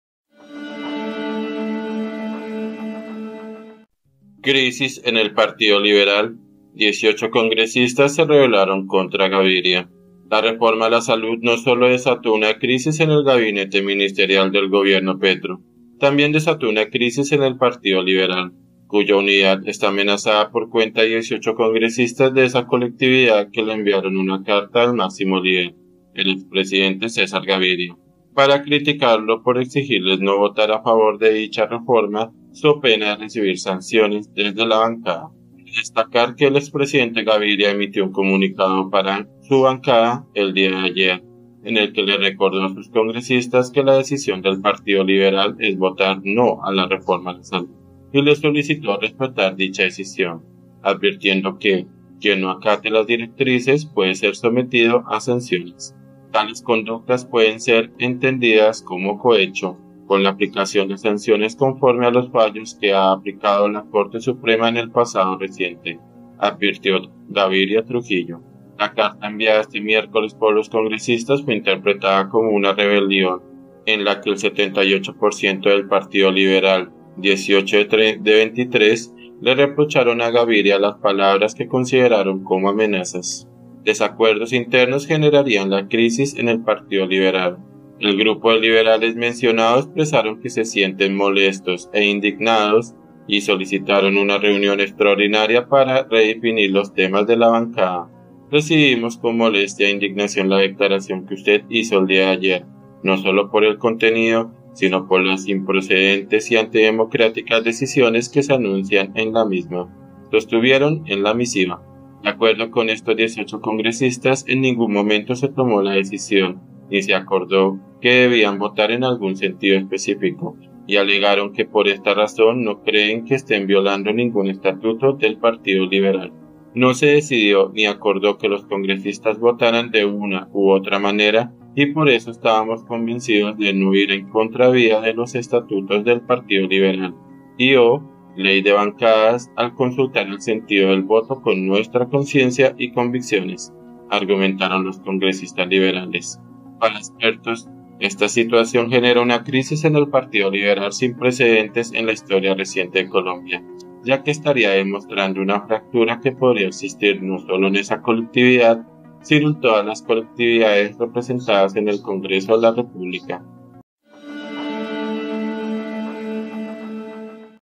🔴 CRISIS SIN PRECEDENTES EN LA HISTORIA DEL PARTIDO LIBERAL
La reforma a la salud no solo desató una crisis en el gabinete ministerial del Gobierno Petro; también desató una crisis en el Partido Liberal, cuya unidad está amenazada por cuenta de 18 congresistas de esa colectividad que le enviaron una carta al máximo líder, el expresidente César Gaviria, para criticarlo por exigirles no votar a favor de dicha reforma so pena de recibir sanciones desde la bancada.
Fuente IFM NOTICIAS